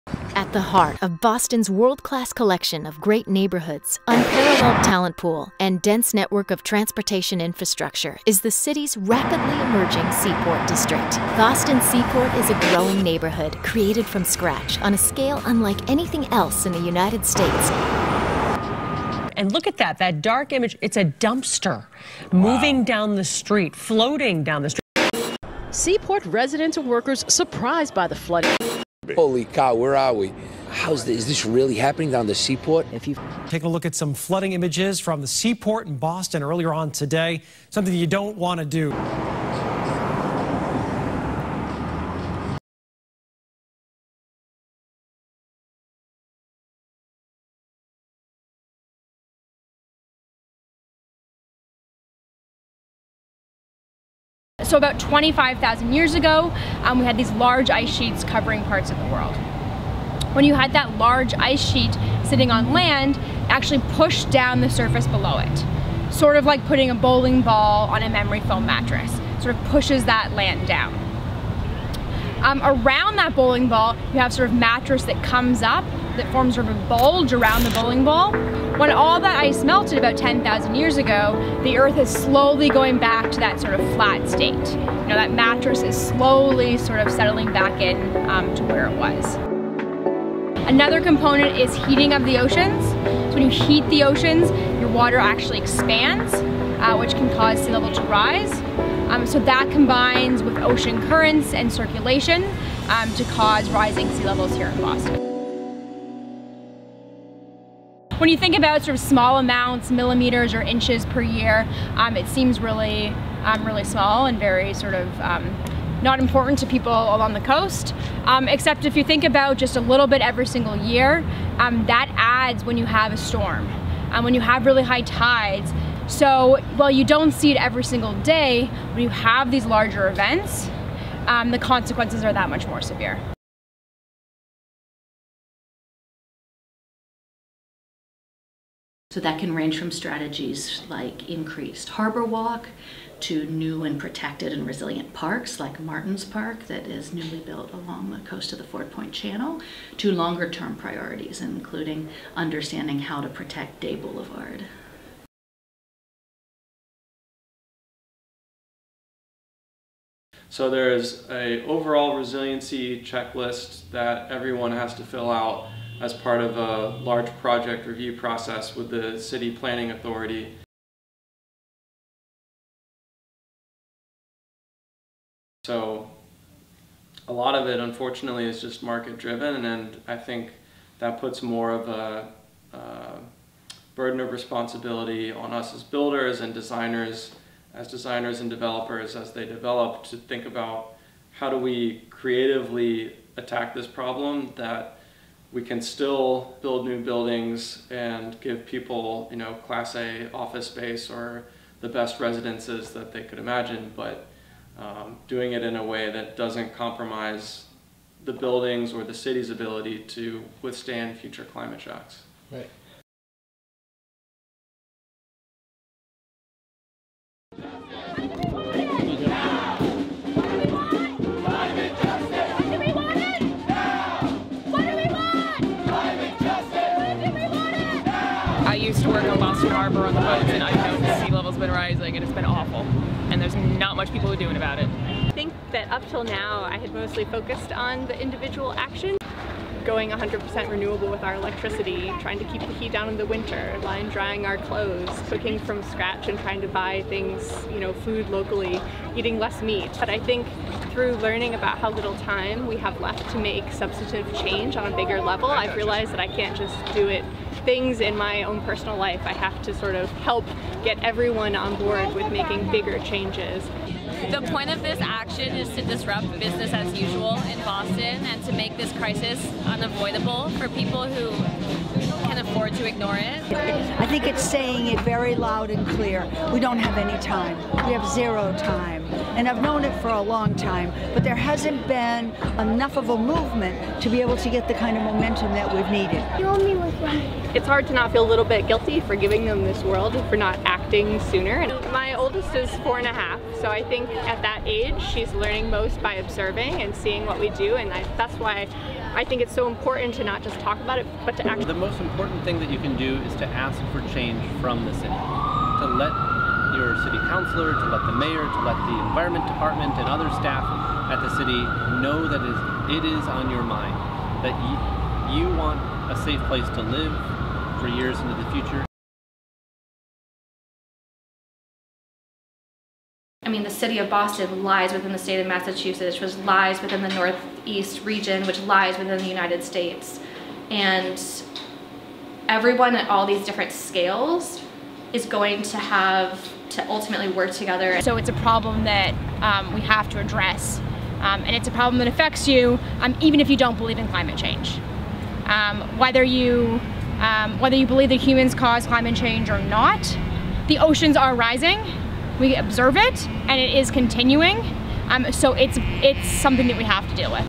SEAPORT DOCUMENTARY VIDEO GROUP 1
This is a documentary about rising sea levels in Seaport.